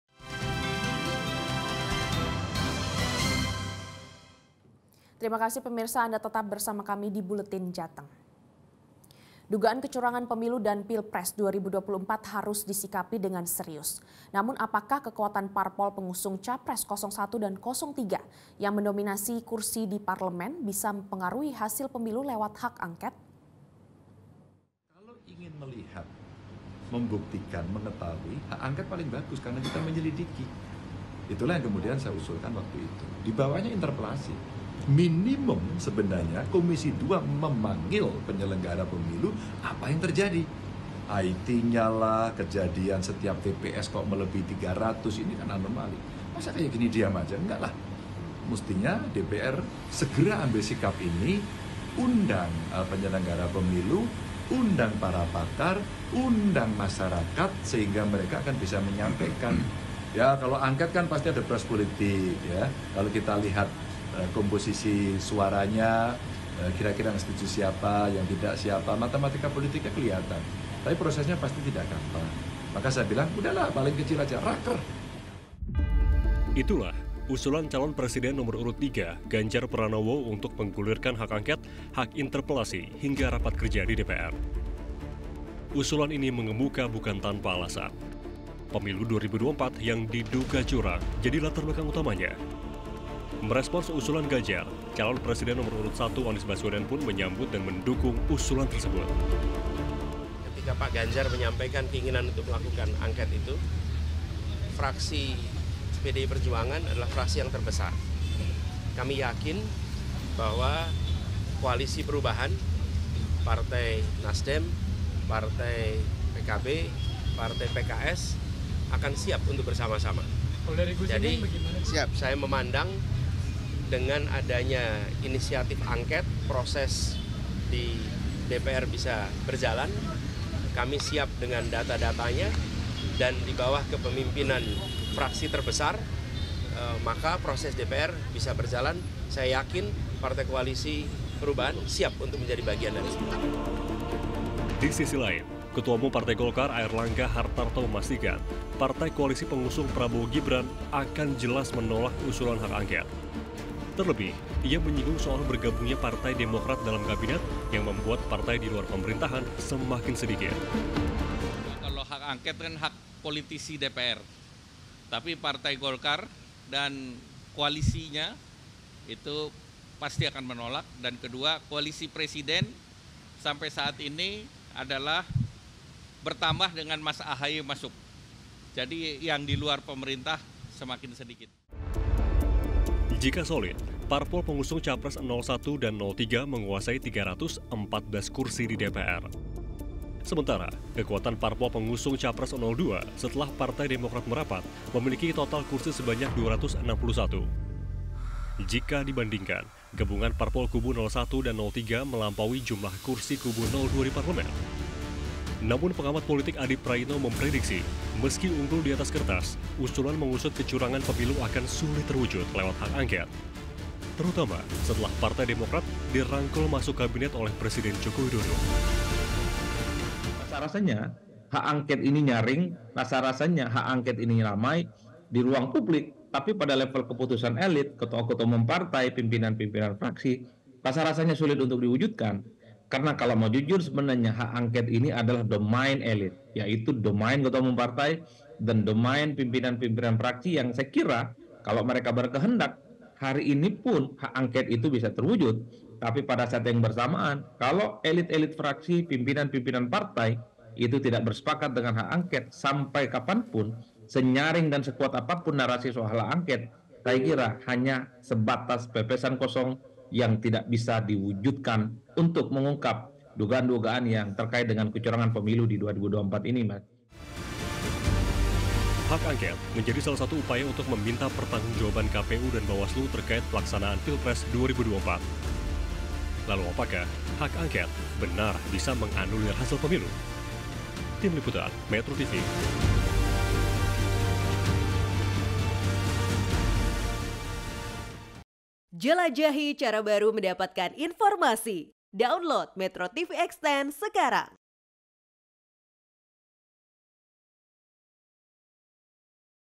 Hak Angket Bergulir, Hasil Pemilu Bisa Dianulir?
METRO TV JATENG & DIY, Dugaan kecurangan Pemilu dan Pilpres 2024 harus disikapi dengan serius. Namun apakah, kekuatan parpol pengusung Capres 01 dan 03 yang mendominasi kursi di parlemen bisa memengaruhi hasil pemilu lewat hak angket?